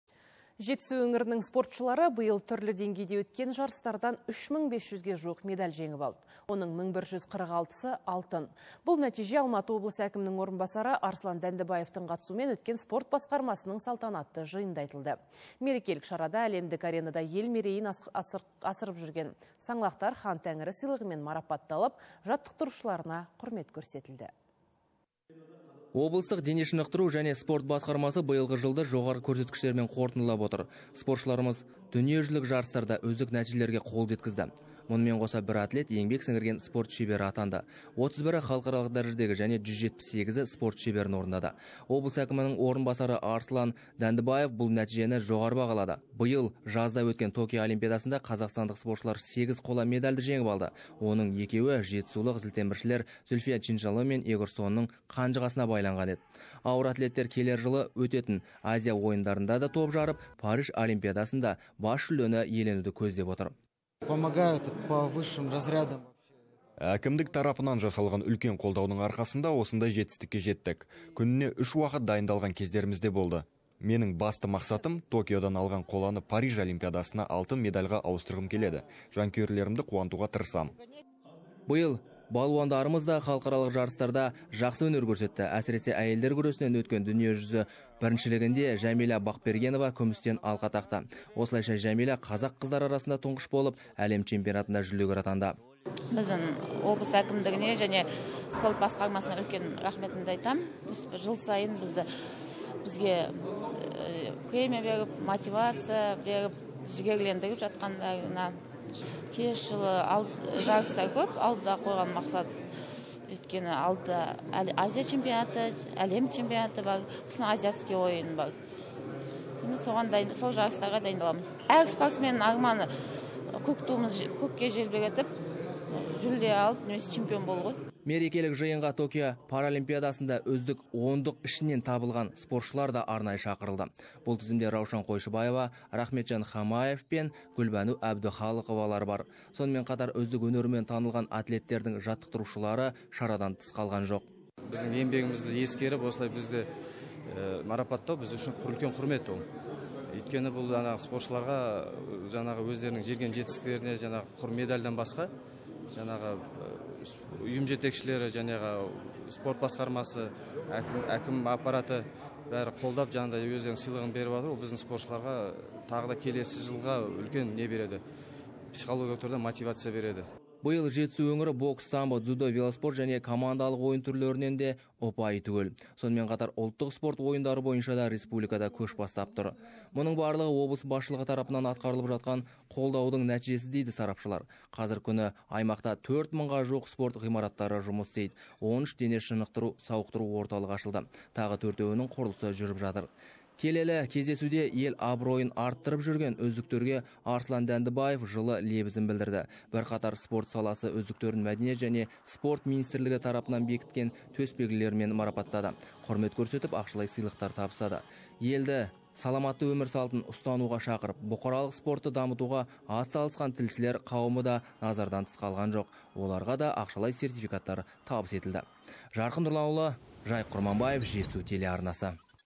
"Хан тәңірі" сыйлығы табысталды
Жетісу өңірінің спортшылары биыл түрлі деңгейде өткен жарыстардан  3500-ге жуық медаль жеңіп алды. Оның  1146 алтын. Бұл нәтиже Алматы облысы әкімінің орынбасары Арсылан Дандыбаевтың қатысуымен өткен спорт басқармасының салтанатты жиынында айтылды. Мерекелік шарада әлемдік аренада ел мерейін асырған саңлақтар "Хан тәңірі" сыйлығымен марапатталып, жаттықтырушыларына құрмет көрсетілді.